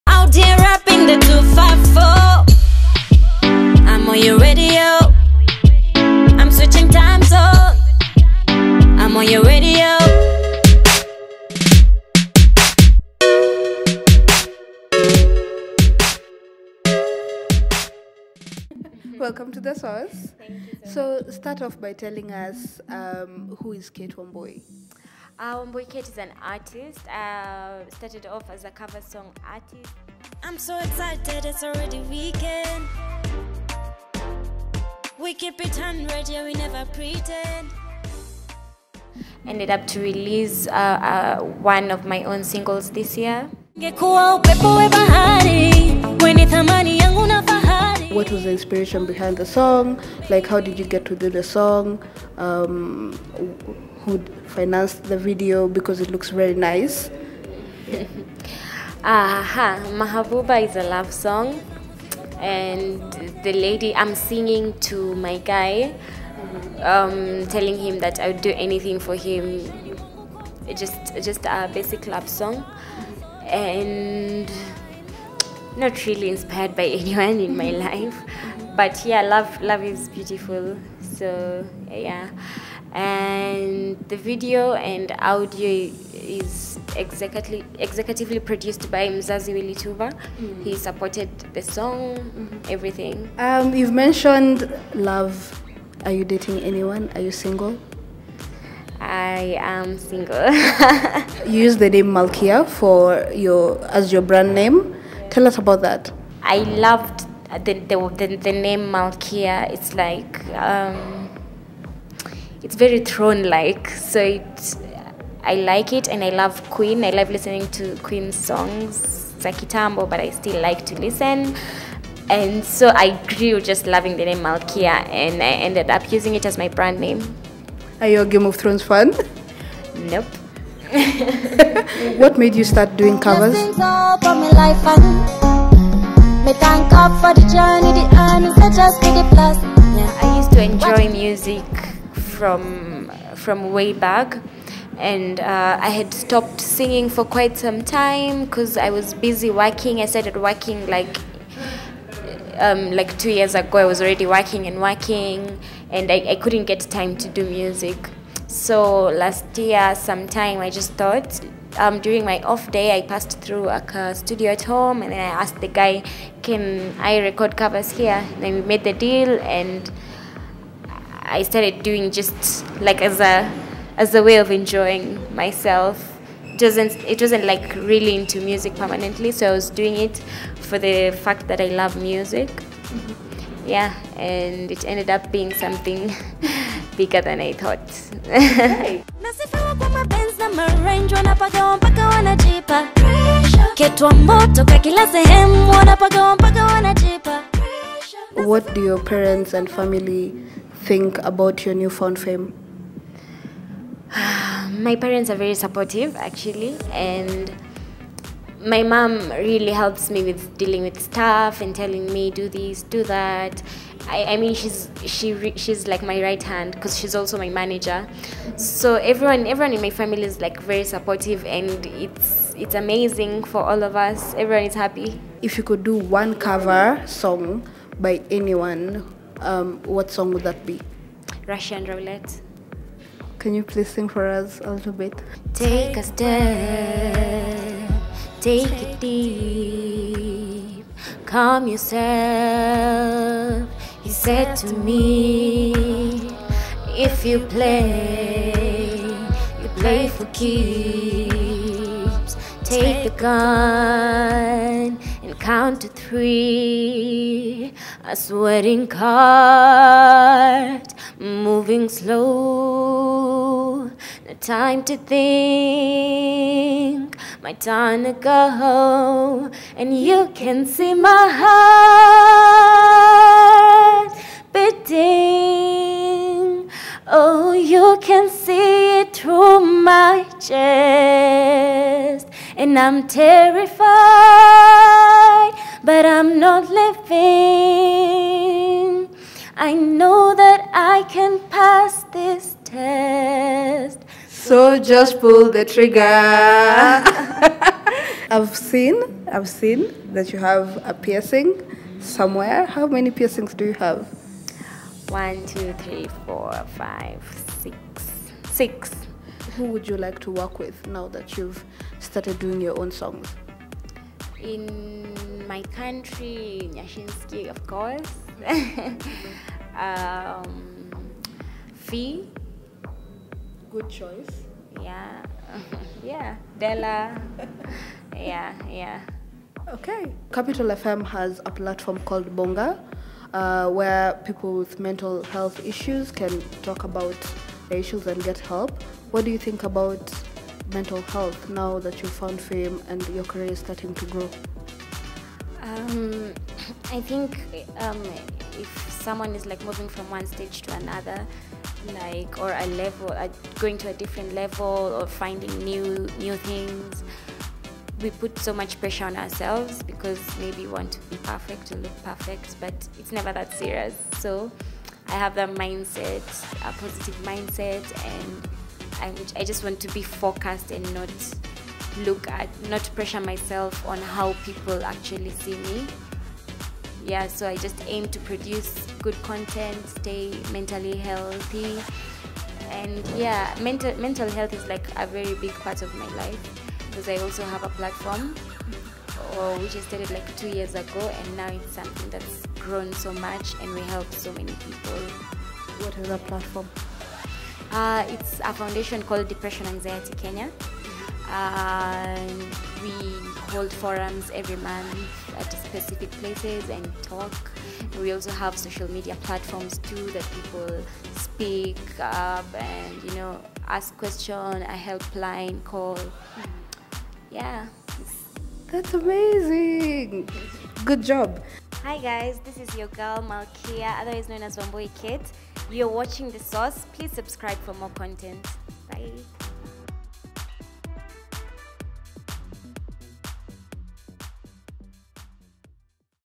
Upcoming artiste Malkia talks about being a cover artiste and growing her craft | The Sauce
Upcoming artiste Malkia talks about being a cover artiste and growing her craft